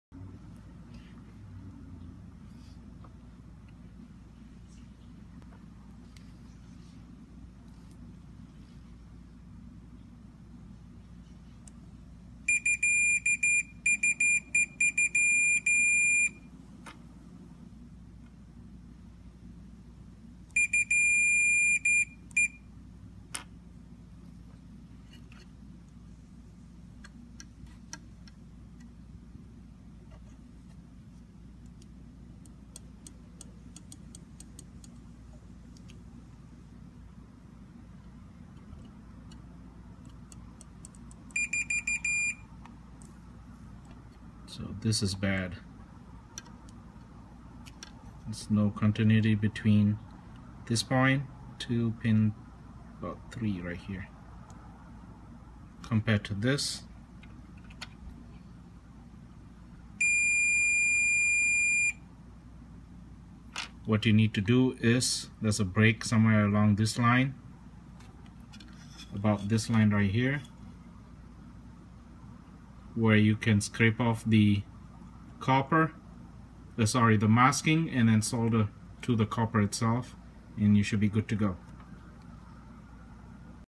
Vortex LED trace troubleshooting
Here is what you need to look for the LED trace break.